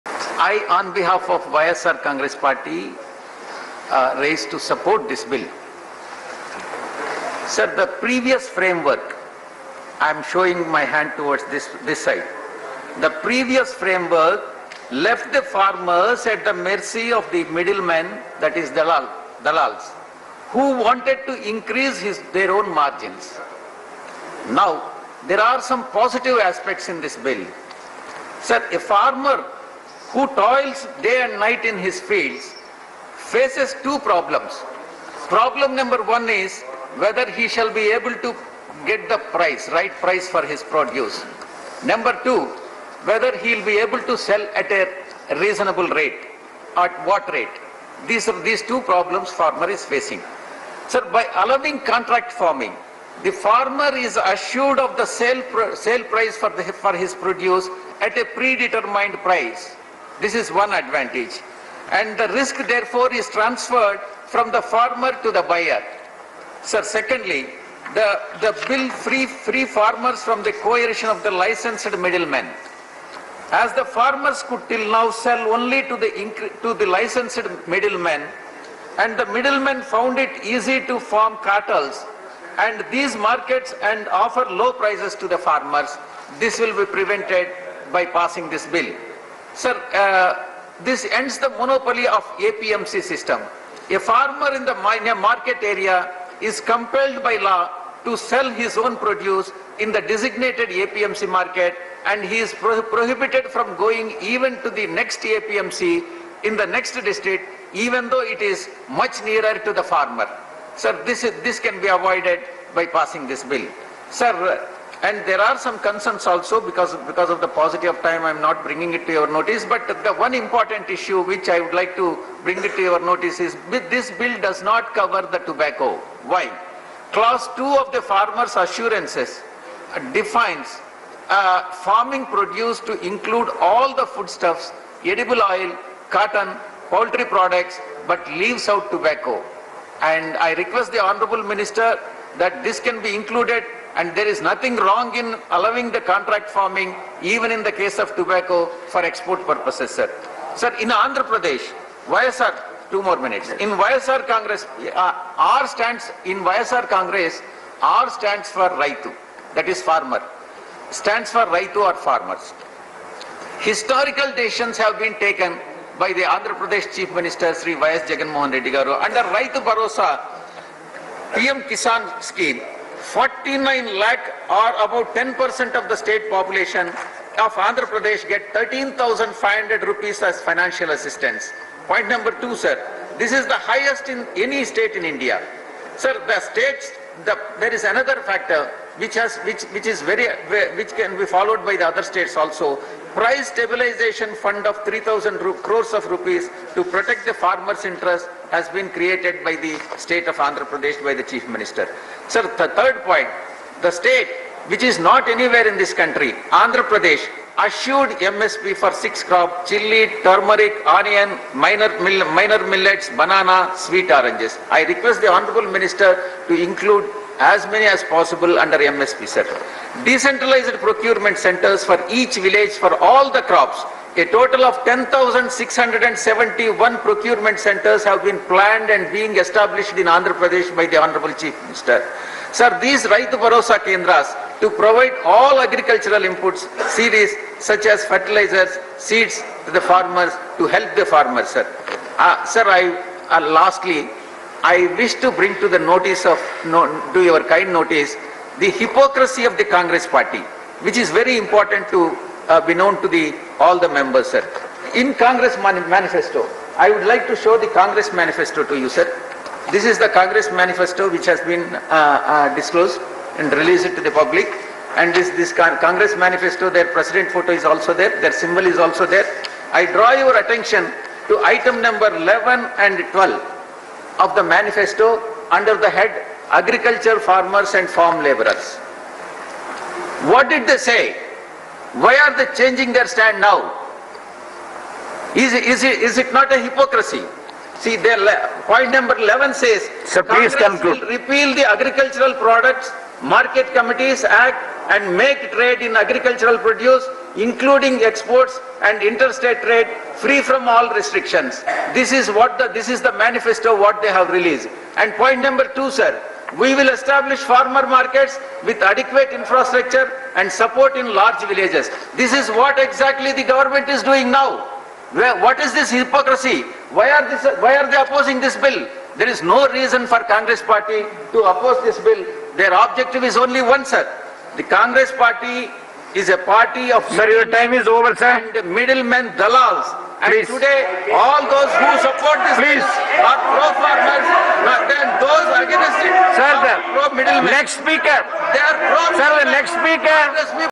YSR Congress MP Vijayasai Reddy Support Agriculture Bill | V6 News
The YSR Congress has supported the Agriculture Bill. Party MP Vijayasai Reddy said, 'The former government used to support middlemen. Farmers were forced to sell their products to licensed middlemen and their cartels. '